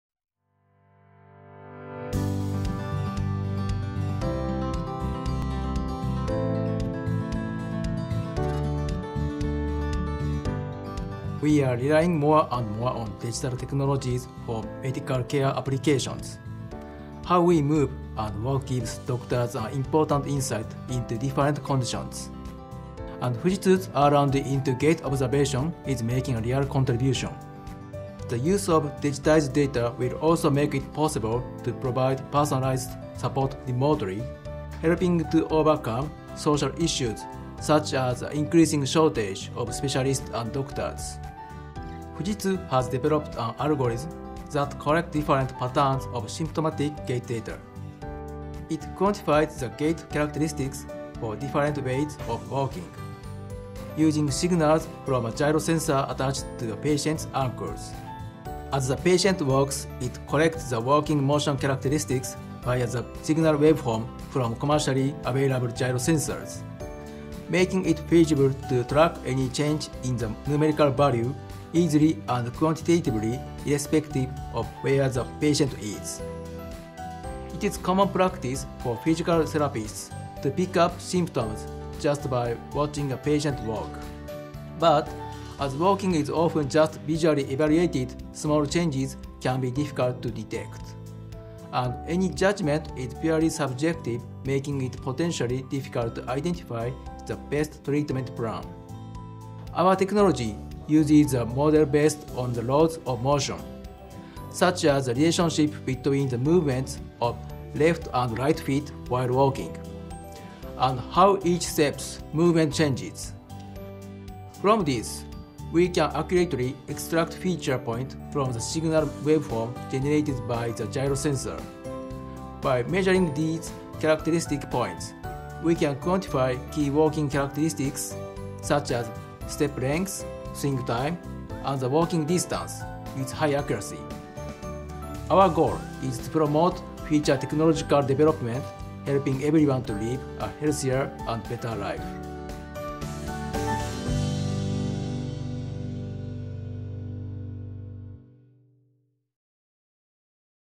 [VDC]Introducing Digitization Technology for Walking Characteristics
Medical professionals can identify the symptoms of patients by observing their way of walking. However, it is difficult to digitize symptoms as there are numerous walking characteristics that differ depending on the type and severity of the disease, and as of now, physiotherapists conduct visual inspections in most cases. Now, Fujitsu has developed a technology to automatically and accurately quantify factors such as the swing time and stance time of the right and left leg as well as the difference between the movements of both legs. In the new development, feature points at the time of movement change will be determined using signal waveforms emitted from commercially available gyro sensors attached to the patients' ankles.